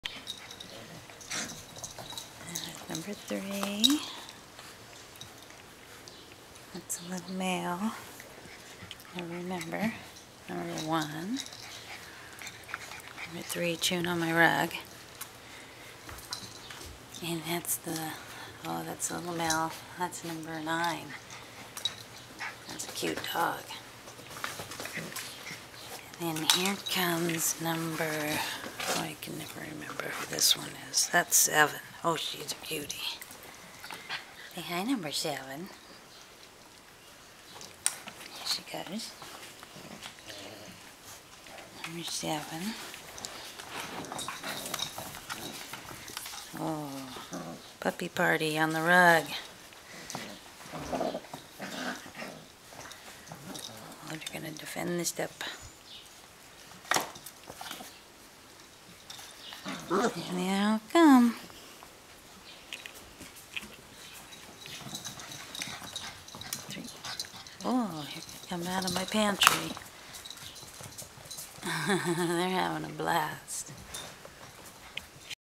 JackienTodds Wirehaired Pointing Griffon Puppies - Puppy party in my kitchen
This is a video of Jackie n Todds puppies playing in my kitchen.  They love my furry rugs and hiding in the pantry!  They were playing hide-n-seek at one point.  Staring in today's video is #'s, 3, 11, 1, 9, 6, and 2 (maybe more but it's hard to keep track!).  Watch #6 jump off the step.  She has no fear!  She even jumps off the deck.  I think she got that skill from her dad, Todd.  He once did a standing jump from the ground into the engine of our truck (thankfully it wasn't running!).